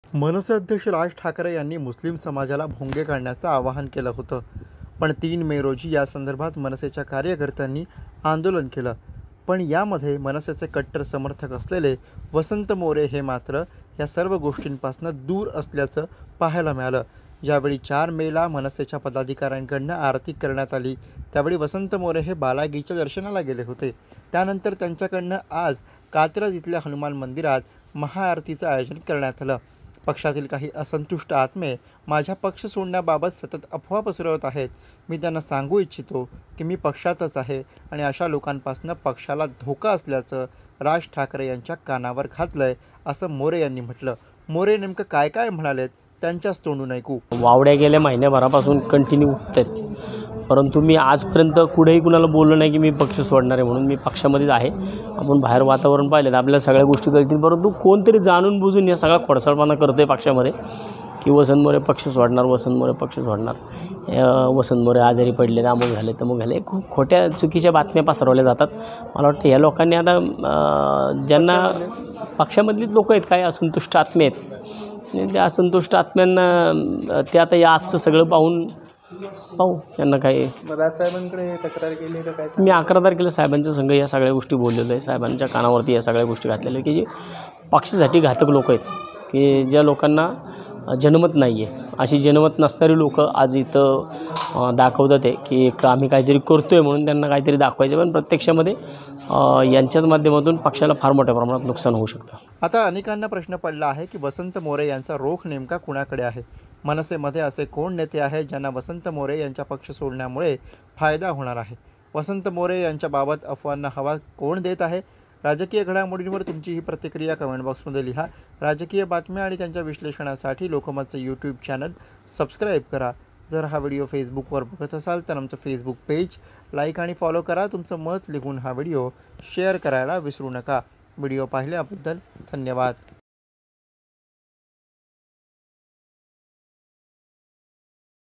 मनसे सोडण्याची चर्चा आणि वसंत मोरेंनी सांगितली 'ती' गोष्ट.. Vasant More to Leave MNS Party
मनसे अध्यक्ष Raj Thackeray यांनी मुस्लिम समाजाला भोंगे काढण्याचे आवाहन केले होते. मात्र 3 मी रोजी यासंदर्भात मनसेच्या कार्यकर्त्यांनी आंदोलनही केले होते.. मात्र यात मध्ये मनसेचे कट्टर समर्थक असलेले Vasant More हे मात्र या सर्व गोष्टींपासून दूर असल्याचे आपल्याला पाहायला मिळाले होते. ज्यावेळी 4 मे ला मनसेच्या पदाधिकाऱ्यांकडून आरती करण्यात आली त्यावेळी वसंत मोरे हे बालाजीच्या दर्शनाला गेले होते. त्यानंतर त्यांच्याकडून आज कात्रज येथील हनुमान मंदिरात महाआरतीचे आयोजन करण्यात आले आहे. पक्षातील काही असंतुष्ट आत्मे माझ्या पक्ष सोडण्याबाबत सतत अफवा पसरवत आहे. मी त्यांना सांगू इच्छितो की मी पक्षातच आहे आणि अशा लोकांपासून पक्षाला धोका असल्याचे आपण राज ठाकरे यांच्या कानावर घातले असल्याचे मोरे यांनी सांगितलय... (Guruprasad VO)